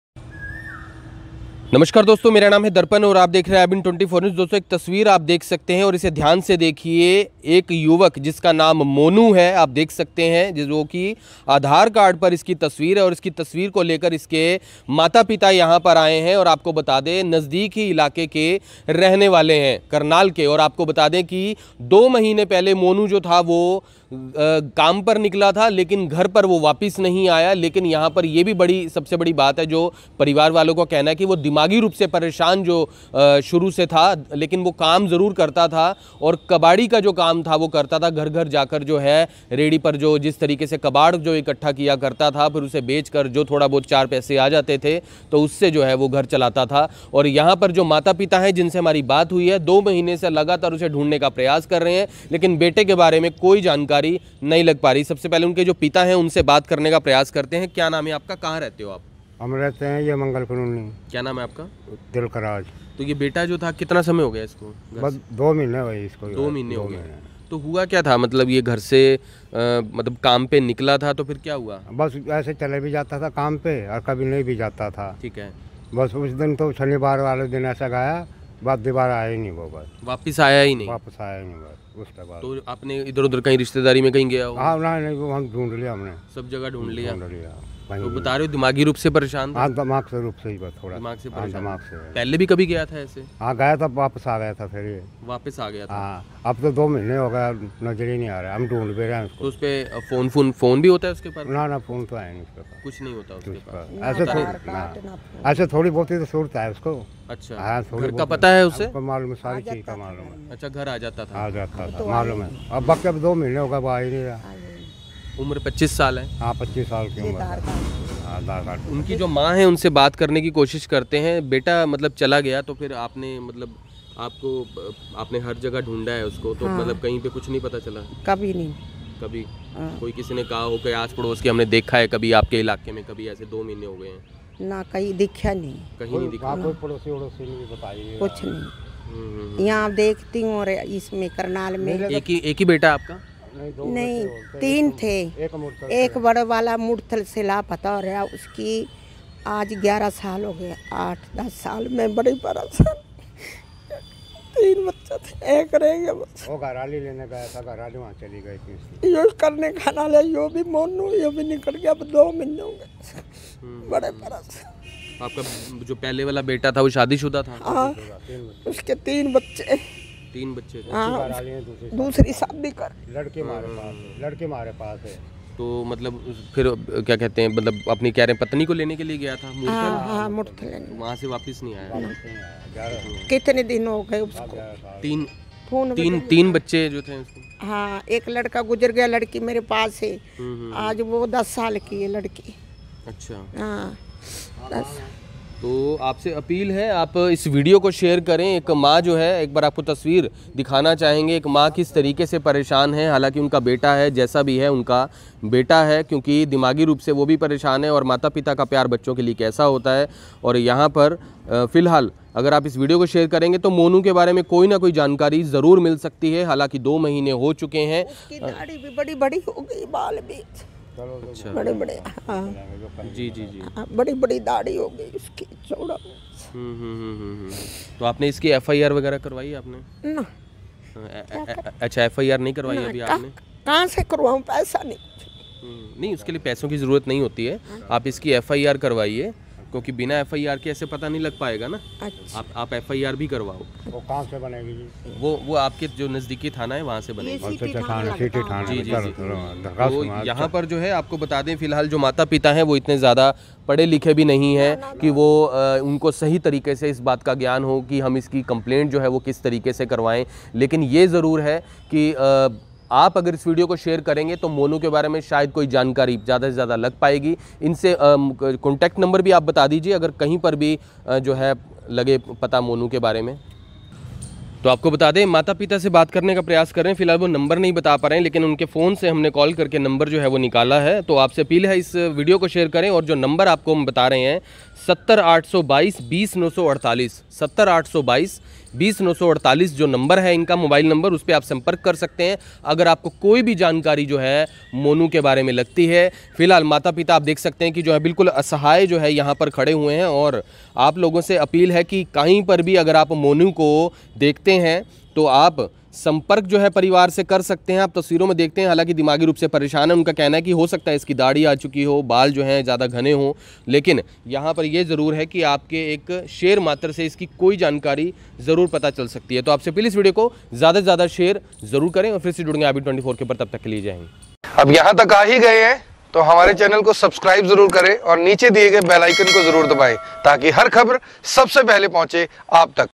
घर से काम पर निकला 25 साल का बेटा नही आया घर वापस, कबाड़ी का काम करने वाला मोनू दिमागी
घर से काम पर निकला 25 साल का बेटा नही आया घर वापस, कबाड़ी का काम करने वाला मोनू दिमागी रूप से भी था परेशान, वीडियो को शेयर कर कोई भी जानकारी मिलने पर दे सूचना : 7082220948